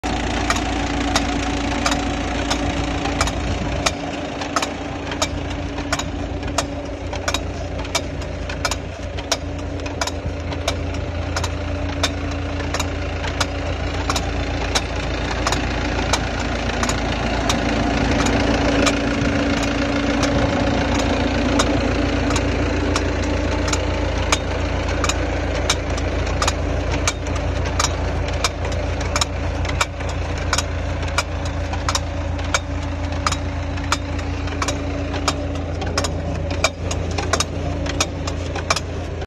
4 Row Hot Pepper Transplanter Machine Supplier for Pepper Seedling Transplanting and Covering Film
4 Row Hot Pepper Chilli Transplanter Machine Supplier for Pepper Seedling Transplanting With Irrigation Drip Tape Layer and Plastic Film Cover.
Driven by tractor PTO Shaft, the machine can plant your Pepper effciently .

HUAYO AGRO
Alice Wang